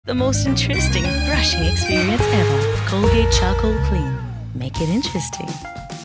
MakeItInteresting with Colgate Charcoal Clean | The bagpiper
Explore the most interesting brushing experience with Colgate Charcoal Clean. Infused with bamboo charcoal and wintergreen mint, it gives your mouth a tingly clean feel every time you brush.